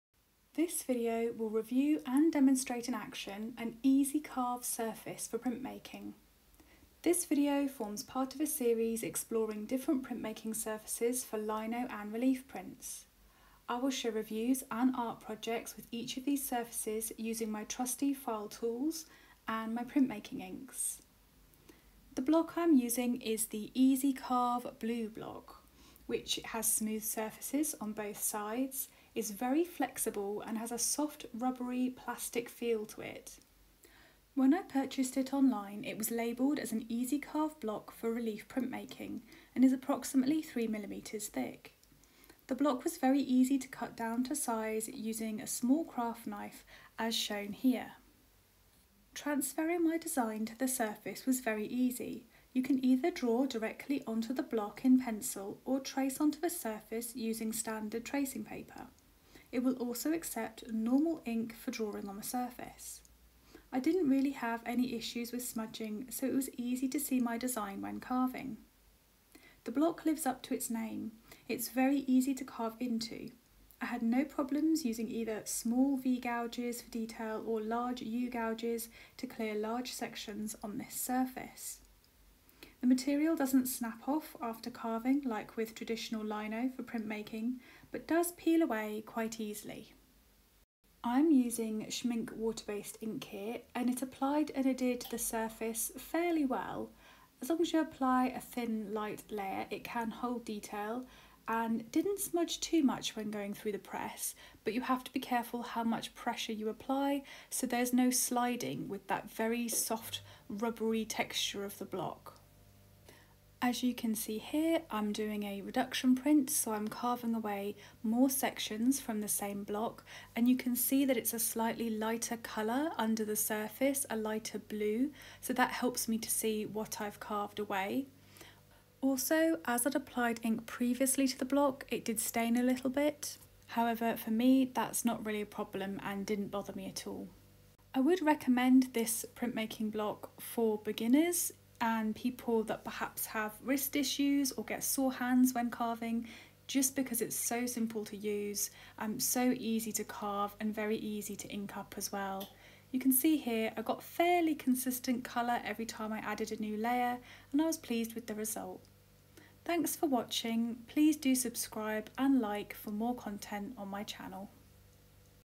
Printmaking with blue easy carve surface - Ep. 5 Relief print surface series
This video reviews a blue easy carve surface for relief printmaking and demonstrates a project carving and inking the product. This video is perfect for beginners to linocut and relief printmaking.